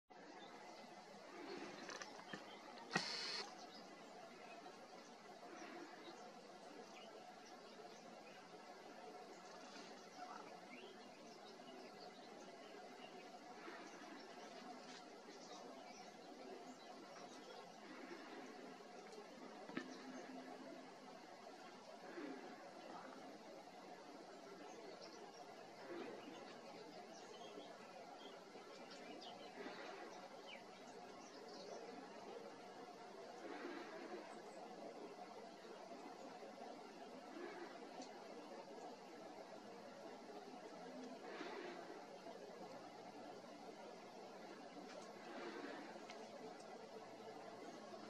Lúgano bebiendo agua condensada por helada en hojas madroño de Hide el venero de los rabilargos
Aquí podemos beber un lugar donde suelen beber las aves, incluso en este caso en que tiene un bebedero al lado, esta hembra de lúgano prefiere beber agua condensada en hojas